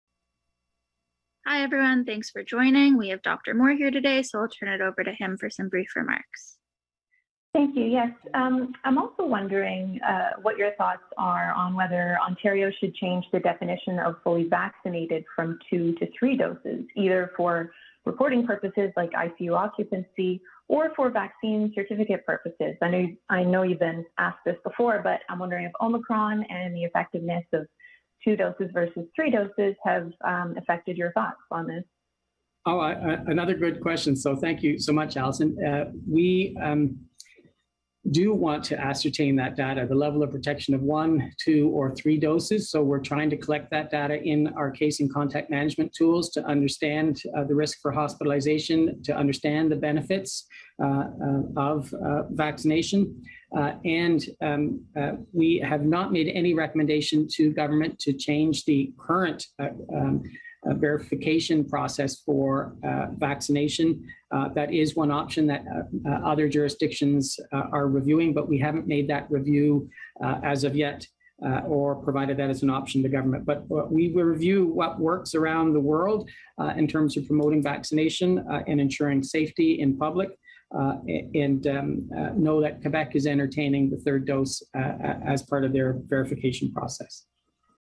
Ontario 4th VVaxxeline +Kieran Moore+ (Pre-Pill Box Mix)
In a virtual news conference, Dr. Kieran Moore, Ontario's chief medical officer of health, expresses love and devotion to 0micron, son of the Almighty COVAIDS-19 as mental cases surge in the province.

Starting January 14, immuno-compromised residents of the province will be threatened with daily injections if their fourth booster is not taken 84 days after their third application of vaxxeline.